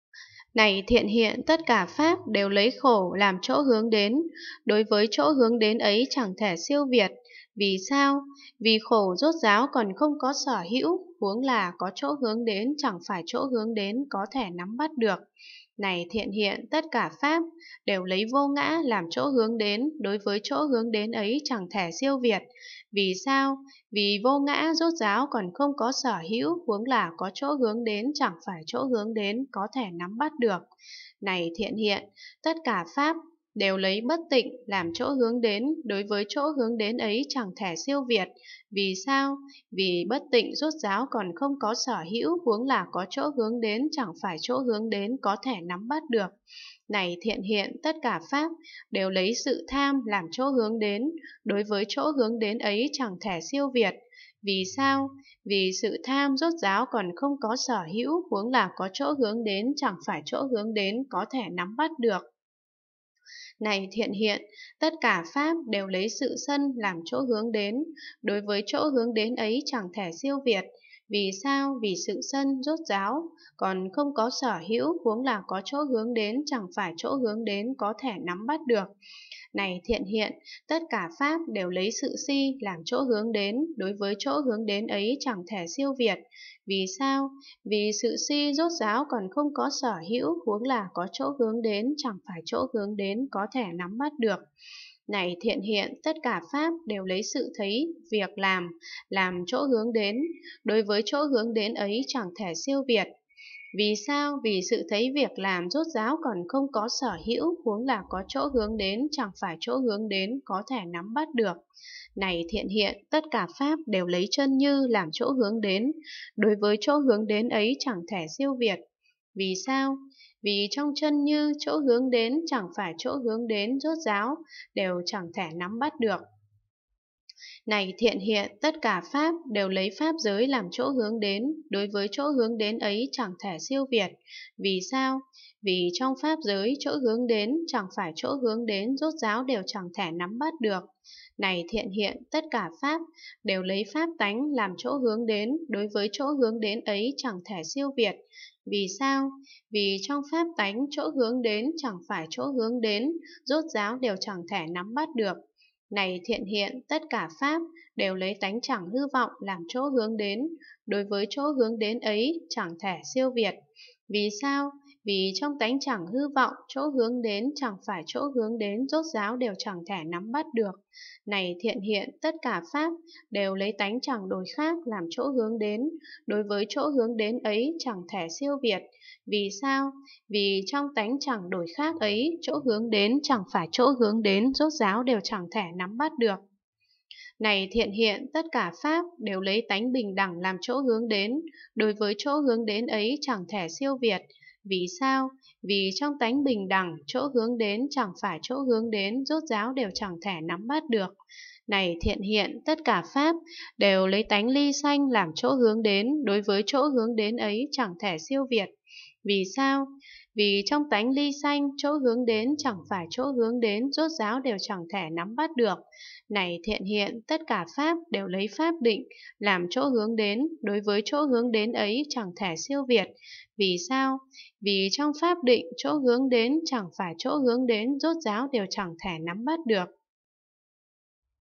Quyển 316A: Kinh đại bát nhã ba la mật đa - Phẩm chân thiện hữu (rất hay) (316/600) (5)
Phẩm 45: Phẩm chân thiện hữu
Hán dịch: Pháp sư Huyền Trang
Việt dịch: Hoà thượng Trí Nghiêm
Giảo chính: Hoà thượng Quảng Độ